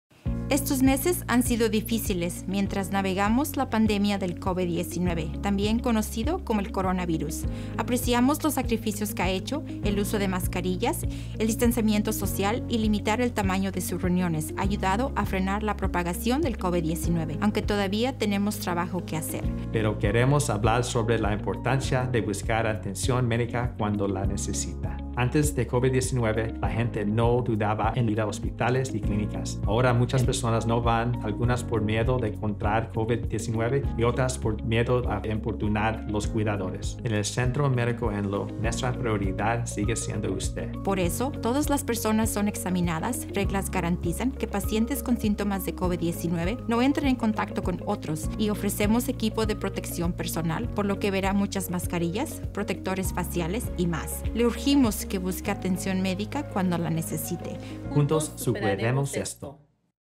Superaremos Esto Junto
¡Busque atención medica cuando la necesite para evitar situaciones peligrosas! Antes del COVID-19, también conocido como el coronavirus, la gente no dudaba en ir a hospitales y clínicas, pero esto ha cambiado. Aprenda más y vea lo que el Centro Médico Enloe está haciendo para mantenerlo seguro. Juntos, superaremos esto.